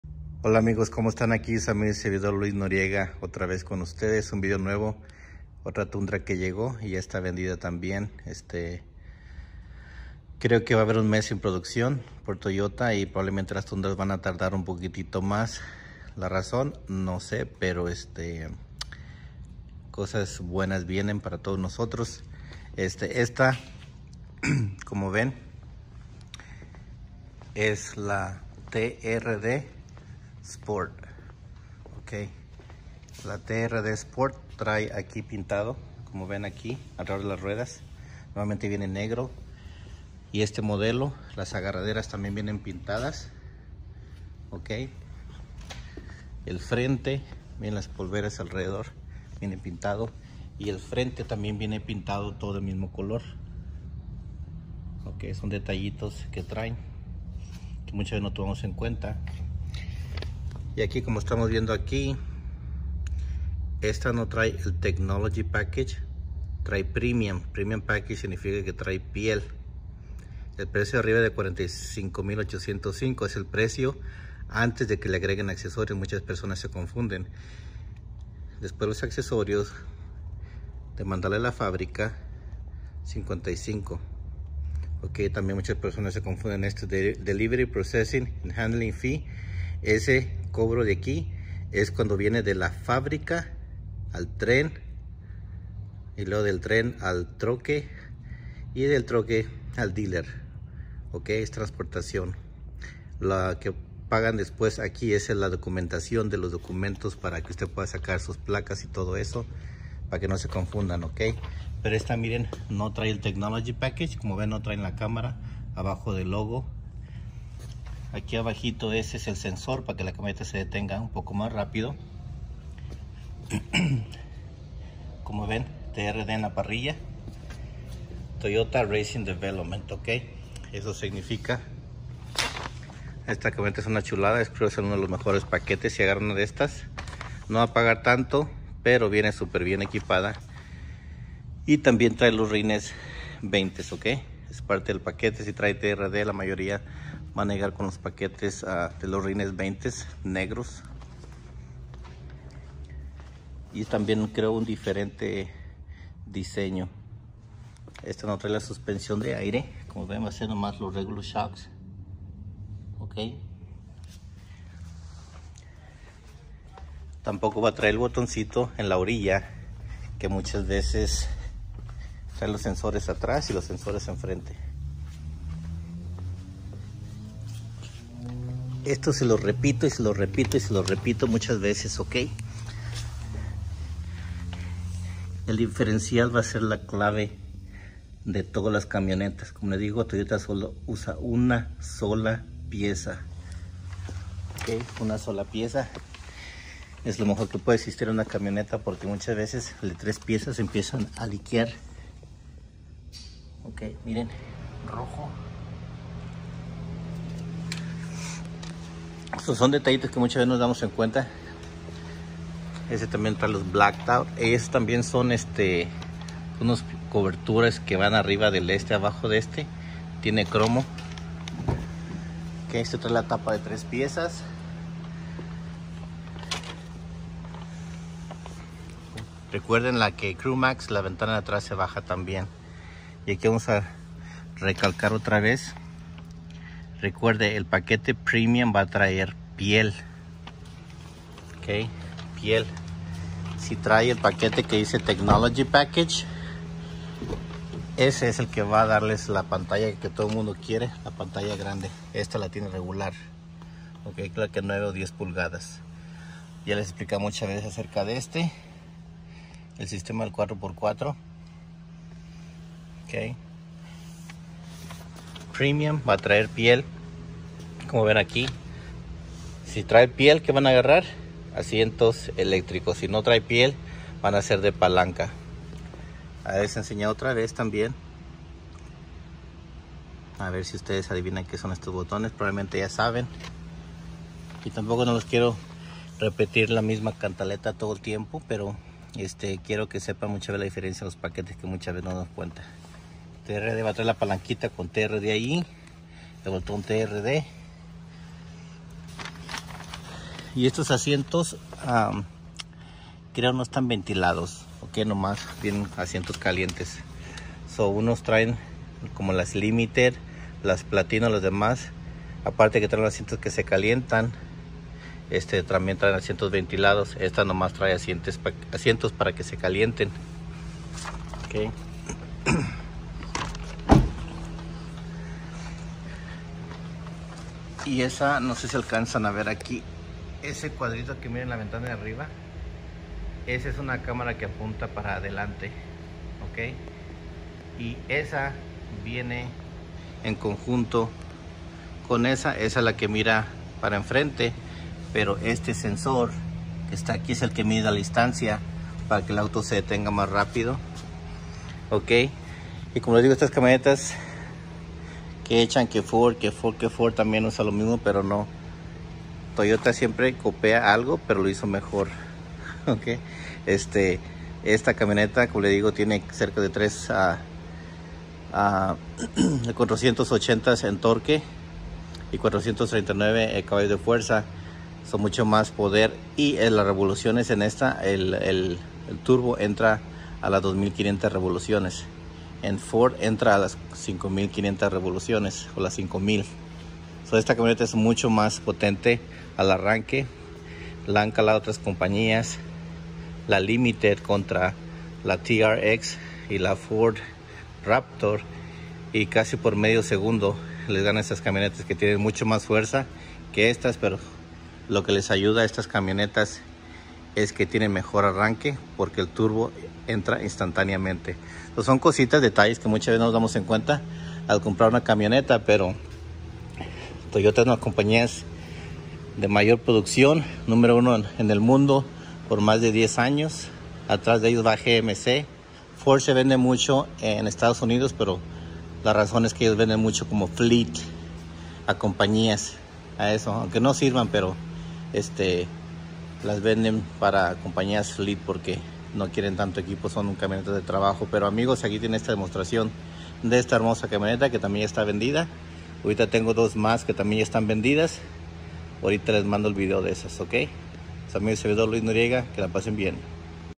Tundra 2022. TRD Sport. Premium. Que trae Lo Premium?
Esta es una TRD Sport. Premium. Con piel. Pantalla normal. Rines 20 que ya los traen la sport. Es parte del paquete.    Si no trae el technology package. No va traer la pantalla de 14”.
La Sr5 con off road. No va sería pintada en el trim. Que va alrededor de la rueda en el la polvera